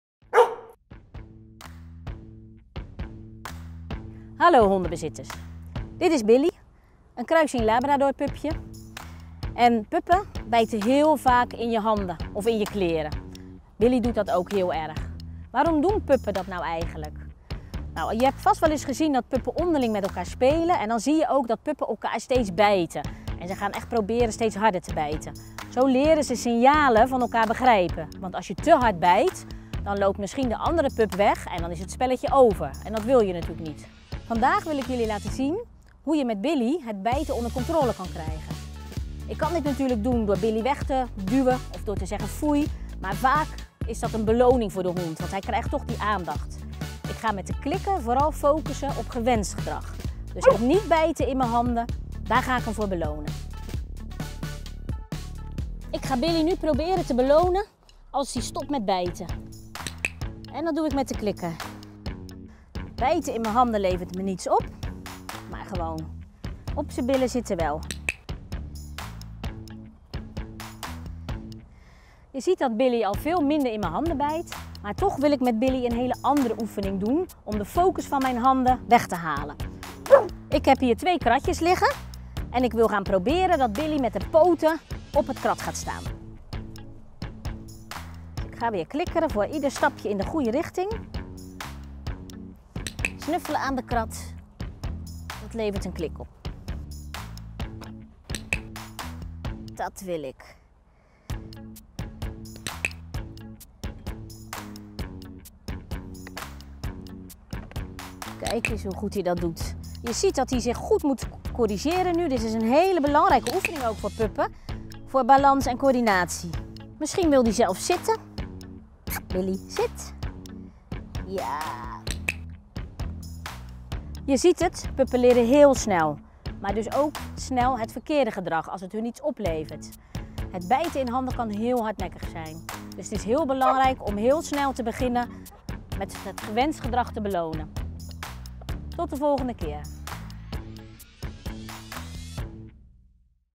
Pup Billy leert happen af │ Lara Leert – Seizoen 2, Les 3
In deze video gaat hondentrainster Myrna aan de slag met een vriendinnetje van hond Lara: Pup Billy! Billy is een gezellige, levendige en vrolijke pup die in haar enthousiasme in je hand of kleren kan happen. Dit is volkomen normaal en speels-lerend gedrag voor een pup, maar het is gedrag dat je als baas liever onder controle hebt, daarom leert Myrna tijdens deze training aan Billy dat niet happen beloond wordt. Ook deze keer werkt Myrna met de clicker.

Clickertraining is een positieve en krachtige trainingsmethode om je hond sneller nieuwe dingen te leren. Train en click je mee?

Billy is, zoals het hoort bij een gezonde pup, een vrolijke en leergierige hond. Ze is een Boulab (kruising Berner Senner / Labrador) pup, komt uit een groot nest en maakt nu onderdeel uit van een fijn gezin. Ze is dol op spelen, kinderen, mensen en alle honden die ze tegenkomt. In je gezicht likken is haar hobby en haar favoriete slaaphouding is op haar rug met alle vier de pootjes in de lucht!

In deze hondentrainingsvideo helpt Billy jouw hond en jou, net als Lara, met een handige leertip!